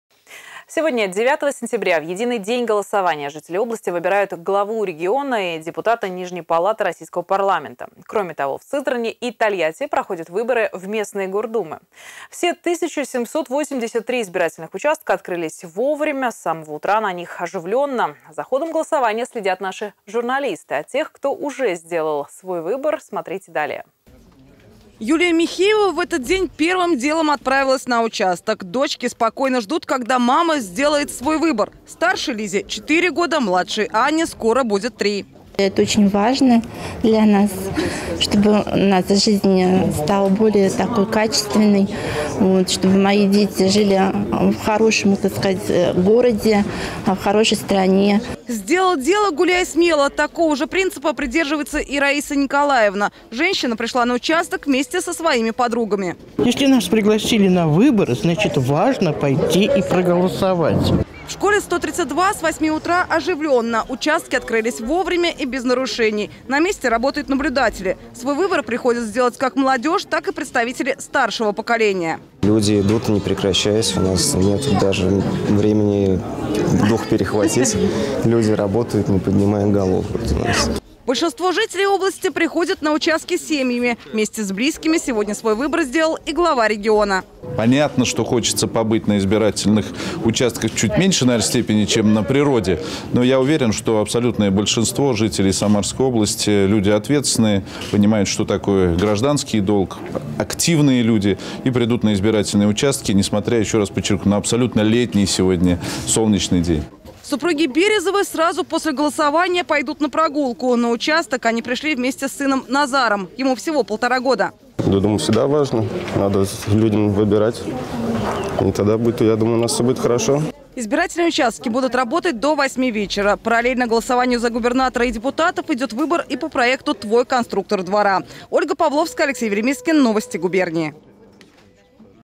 Как проходит единый день голосования в Самарской области
Сегодня, 9 сентября, в единый день голосования, жители области выбирают главу региона и депутата нижней палаты российского парламента. Кроме того, в Сызрани и Тольятти проходят выборы в местные гордумы. Все 1783 избирательных участка открылись вовремя. С самого утра на них оживленно. За ходом голосования следят наши журналисты. О тех, кто уже сделал свой выбор, смотрите далее.